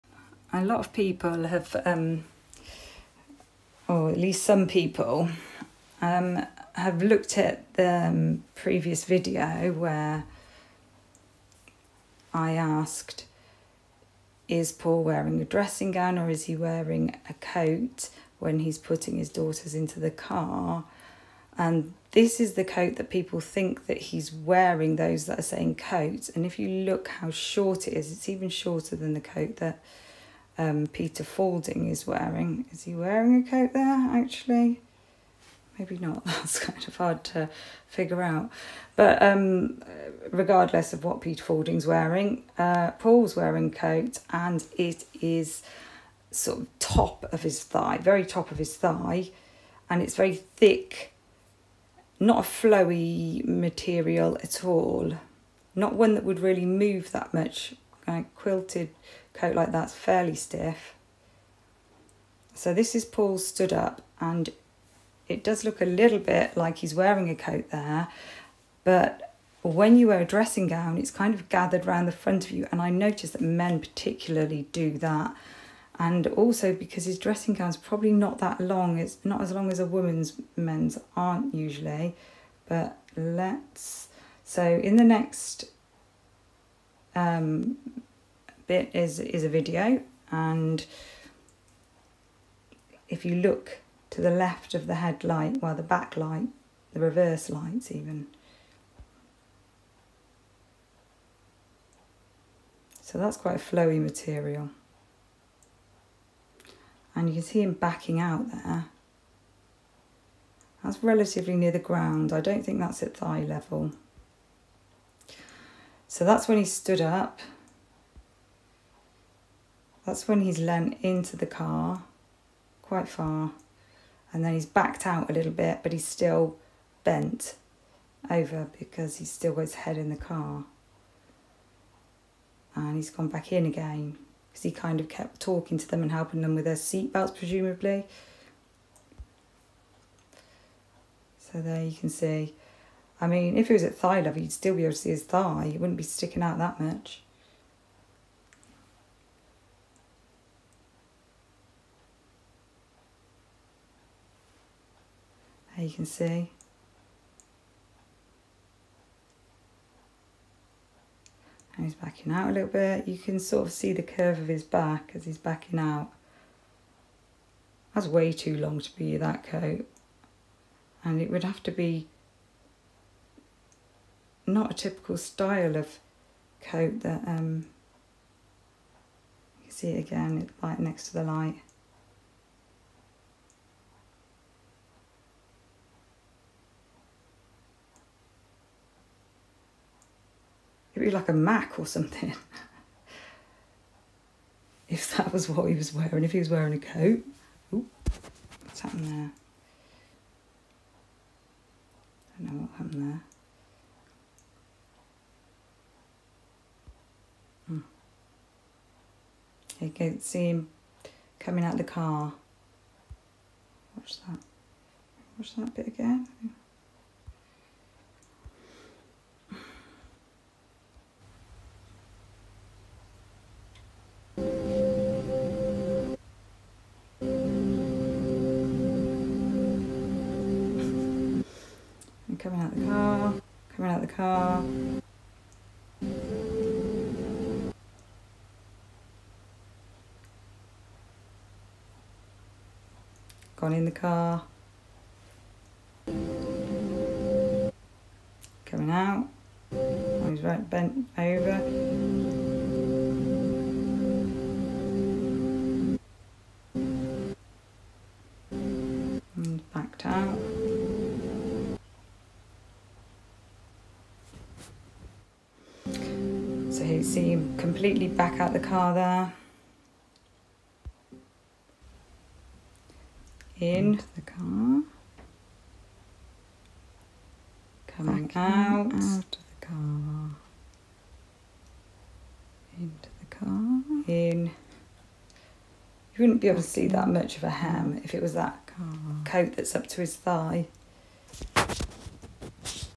The dressing gown
Clearing up some confusion for the less eagle -eyed 🦅

Regardless of what is worn. It doesn’t prove anything either way. One thing that you may not know is that innocent people frequently get facts and events mixed up when they are put on the spot. An interesting study in relation to this phenomenon:

Spoiler alert though… I am not ruling anything out. I just don’t have sufficient evidence to lean in any direction. My strongest instinct is that Lancashire Police did not spring into action and it was only the unwanted publicity that caused them to pull their fingers out despite the disruption of “social media”…  🙄… Give me a break 🤨 …

Keep your eye 👁️ on lancashirepolice if it’s funny business you’re after 🤡